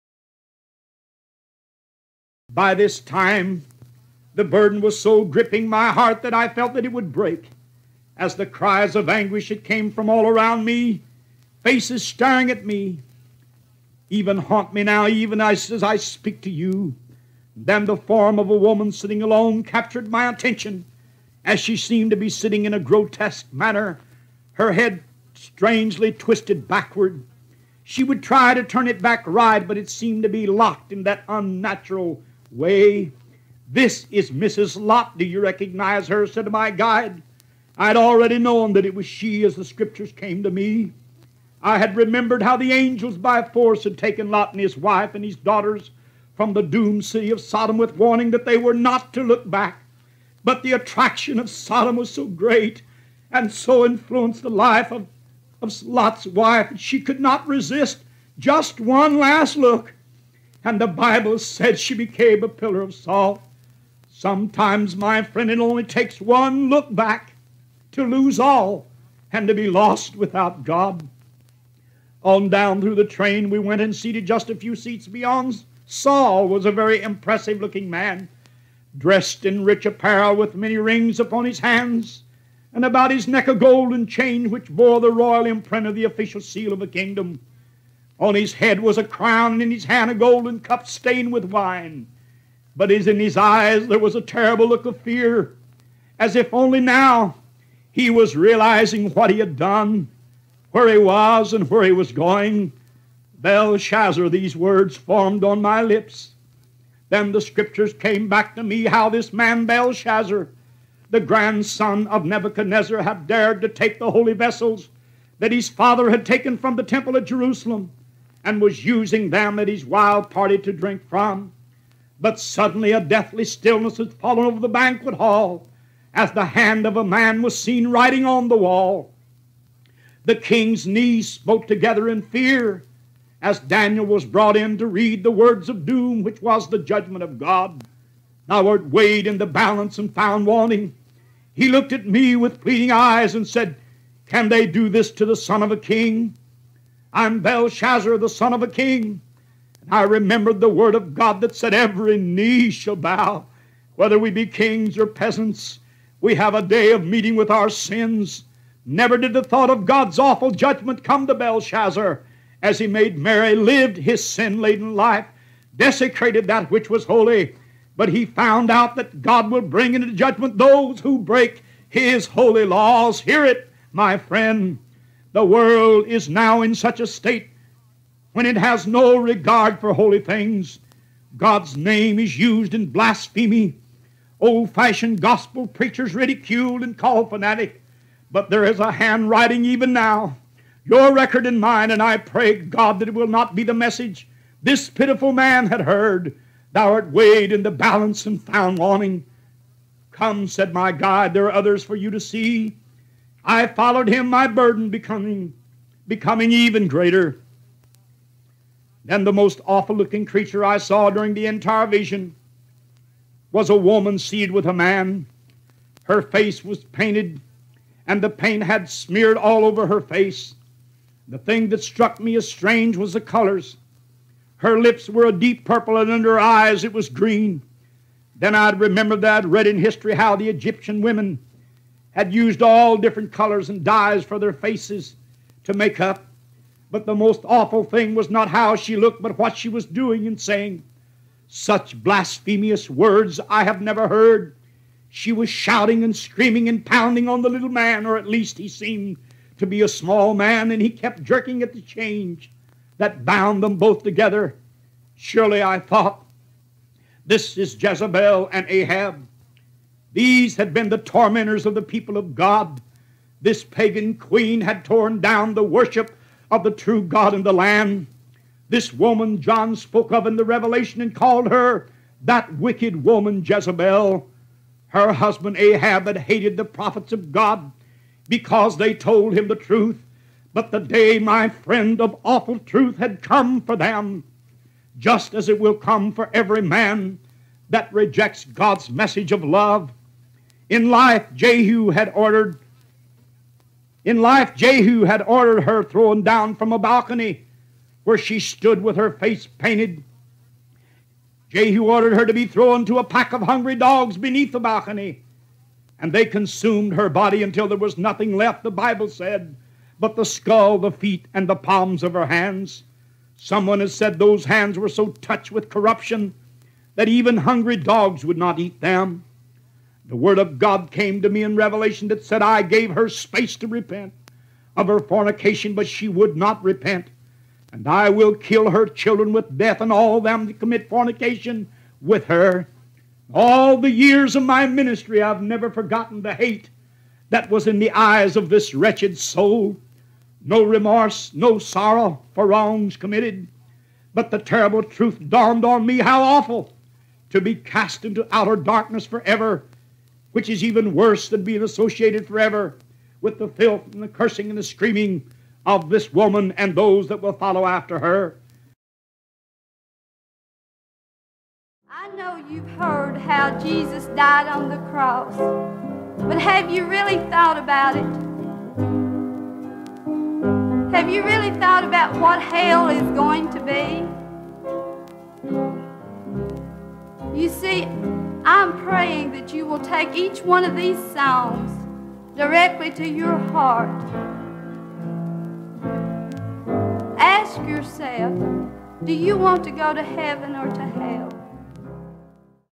H.P. Vibbert - Lot's Wife, Belshazzar And Jezebel On The Hell-Bound Train [1960s Sermon excerpt]
"Lot's Wife, Belshazzar And Jezebel On The Hell Bound Train", excerpt from H.P. Vibbert's sermon "The Hell-Bound Train".